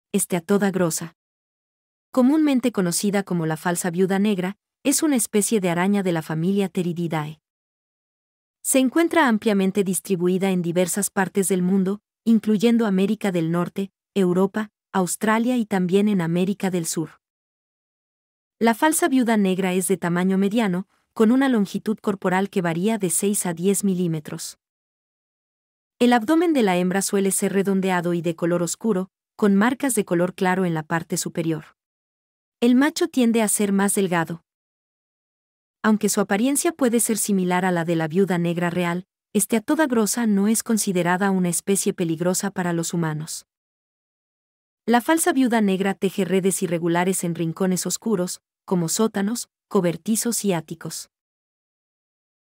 Steatoda grossa, la falsa viuda negra 🕷🕸
Steatoda grossa es una especie de araña conocida como falsa viuda negra. Aunque se asemeja en apariencia a la viuda negra real, no es considerada peligrosa para los humanos y su mordedura rara vez causa complicaciones graves. Son arañas relativamente comunes que se encuentran en varios lugares del mundo, y suelen tejer sus redes en rincones oscuros para capturar insectos pequeños.

Imágenes y video principales tomados por el autor en Chile. Agradecimientos a la araña por haber cooperado para el video.